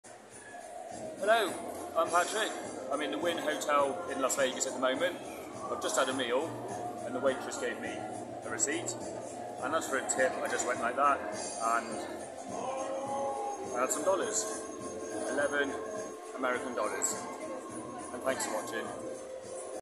Magic with a receipt! In The Wynn Hotel in Las Vegas!
The Wynn Hotel in Las Vegas!
Magician Patrick Aitchison performing an amazing magic effect with a receipt!